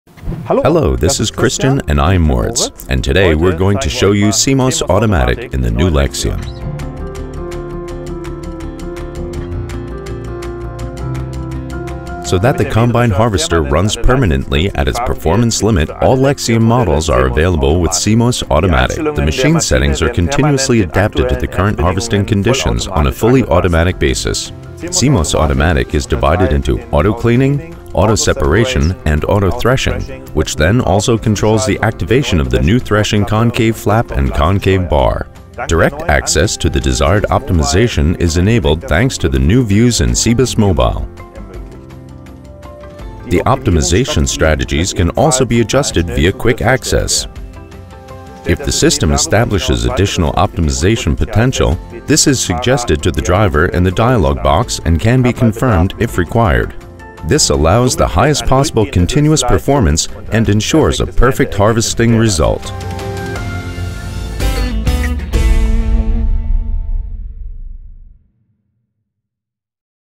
CLAAS LEXION 8000-5000. CEMOS AUTOMATIC.
CEMOS AUTOMATIC continuously – and automatically – adjusts the threshing process (CEMOS AUTO THRESHING), residual grain separation (CEMOS AUTO SEPARATION) and cleaning (CEMOS AUTO CLEANING) to the current harvesting conditions. The intelligent, self-learning system tests various settings in order to optimise the combine harvester right up to its technical performance limits.